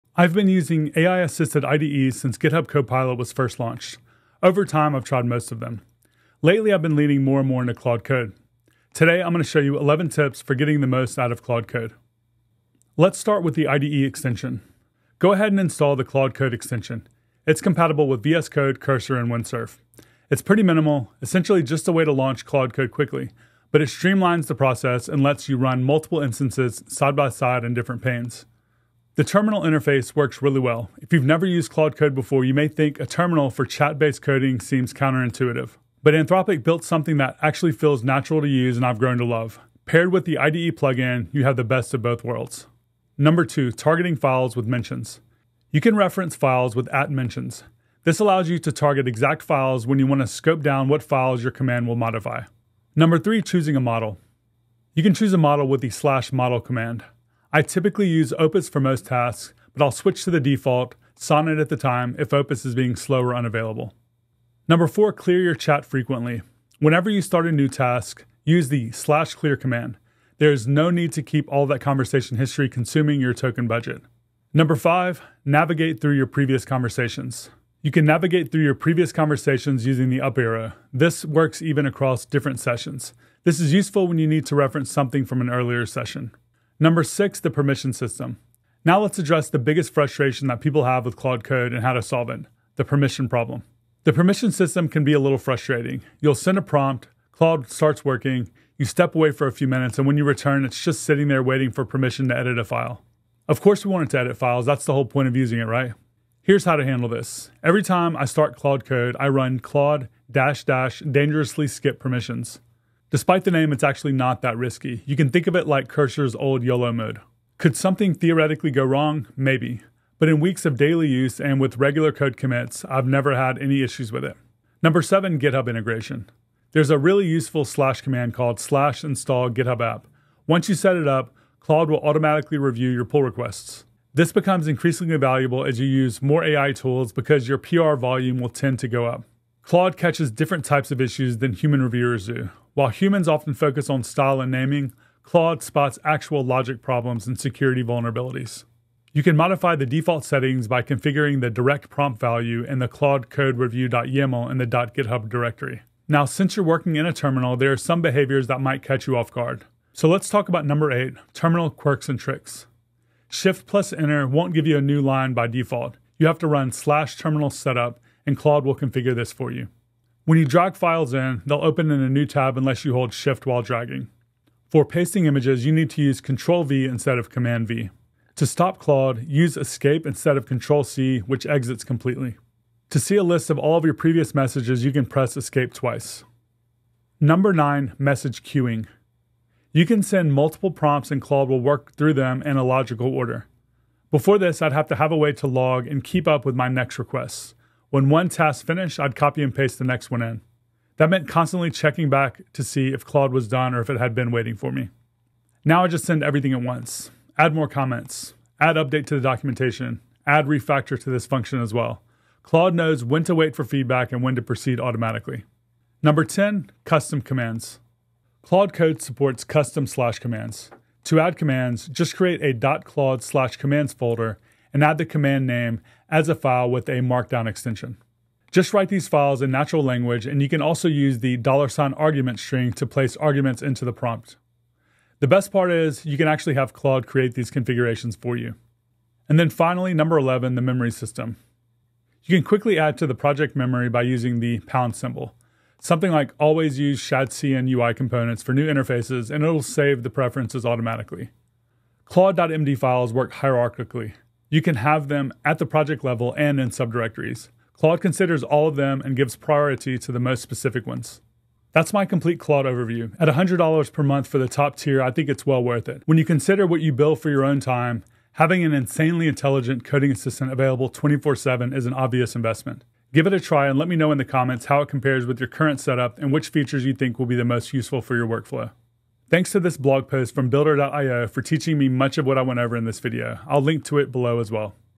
Mastering Claude Code - 11 Tips in 4 Minutes for Vibe Coding Excellence
This tutorial covers the essential Claude Code setup and workflows that maximize productivity for daily development work.

You'll learn the configurations, shortcuts, and advanced features that experienced users rely on.

These techniques help you get the most out of Claude Code.